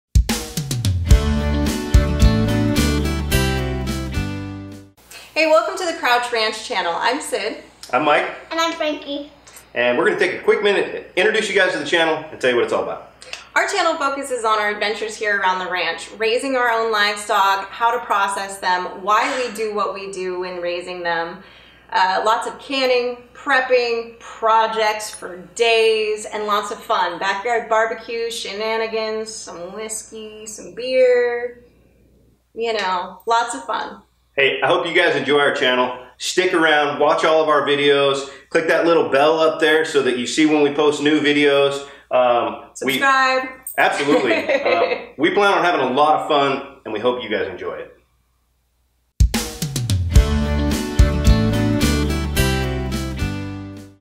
What The Crouch Ranch Channel is about..In under a minute...Ready..GO!
Howdy folks and welcome to the Crouch Ranch.  We hope you like our channel and what we are all about, here is a brief intro in under a minute.